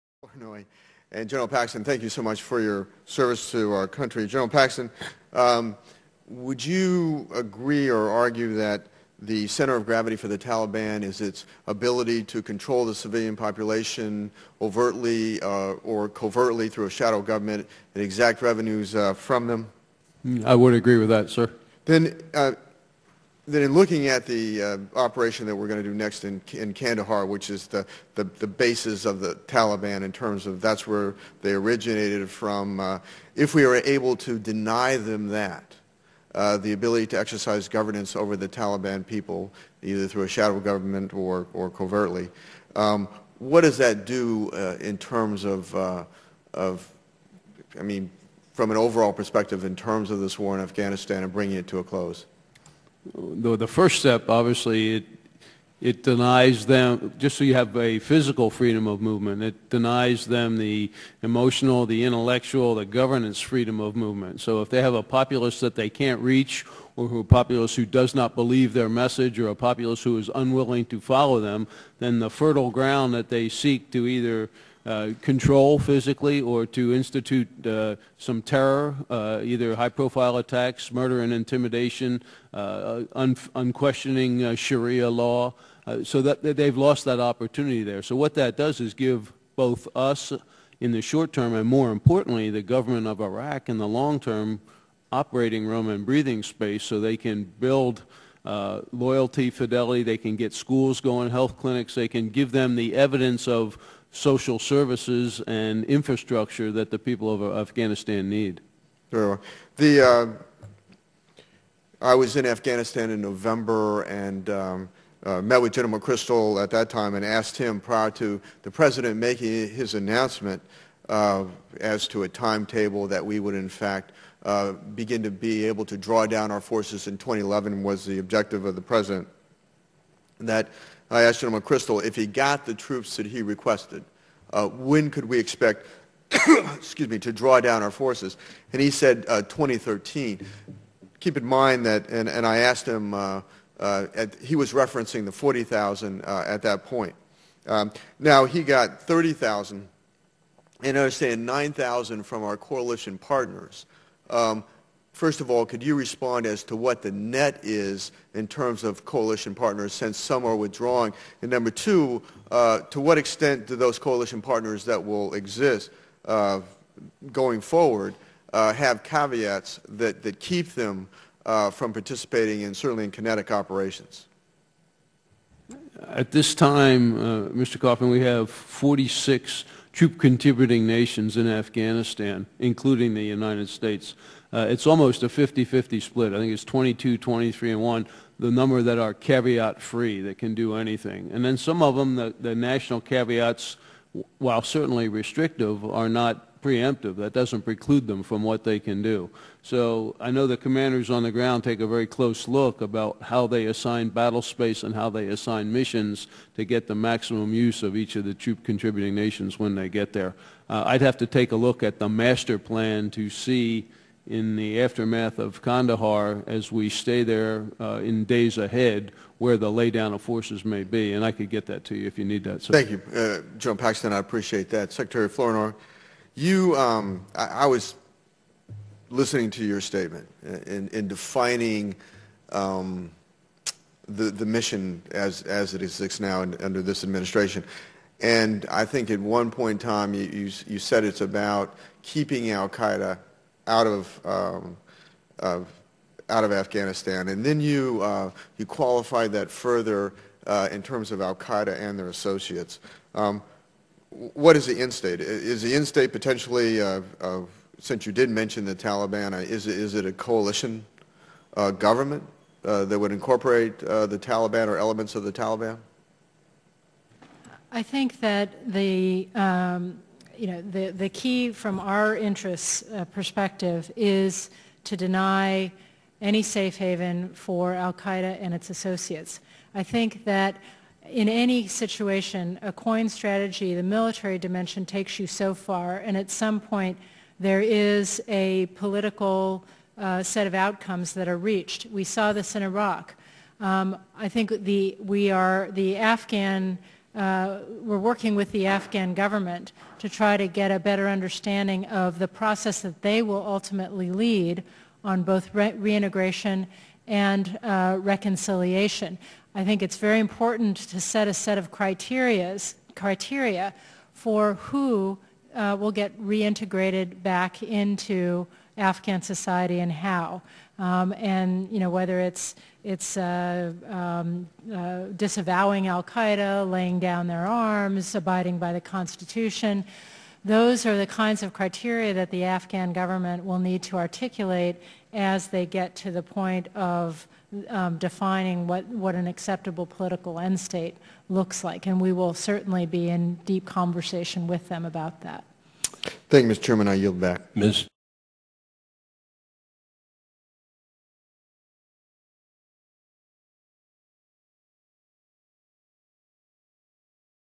Developments in Security and Stability in Afghanistan
Rep. Coffman questions The Honorable Michèle Flournoy, Under Secretary of Defense for Policy, U.S. Department of Defense, and Lieutenant General John M. Paxton Jr., USMC, Director for Operations, J-3, Joint Chiefs of Staff, about the developments in security and stability in Afghanistan.